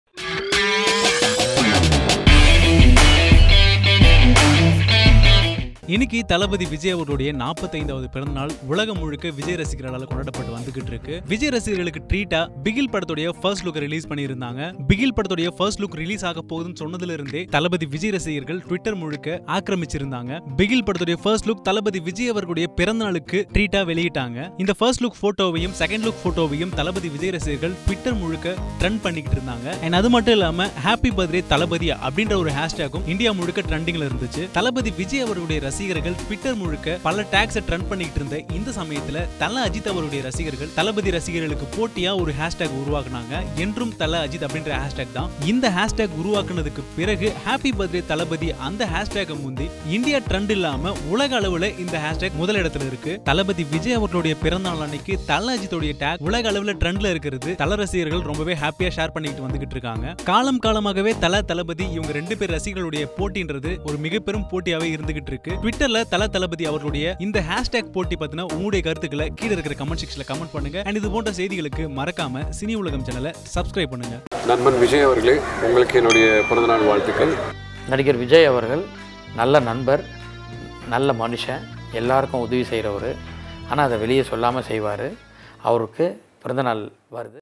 VIJAY பிறந்த நாளில் AJITH ரசிகர்கள் செய்த அட்டகாசம் | Thala VS Thalapathy
This Video Contains About the Thala fans was Creates the tag that என்றும்_தலஅஜித். For the vijay Birthday